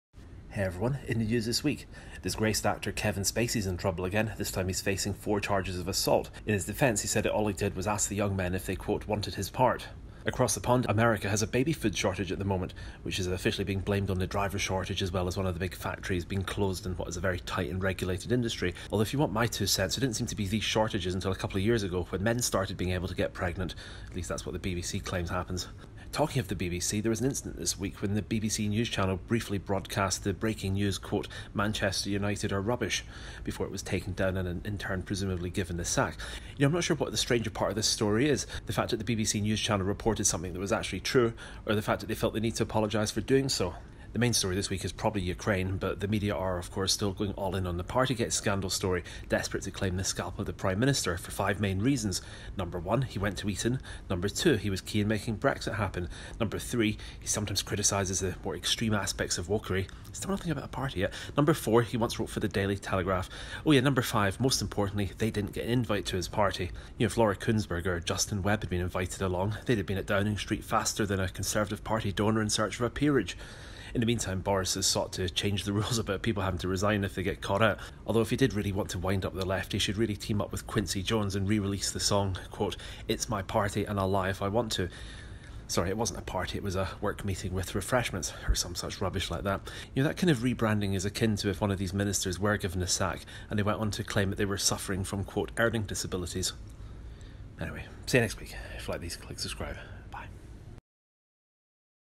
2022 May 29 - Partygate
News summary for a week in which the media kept demanding the PM resign over Partygate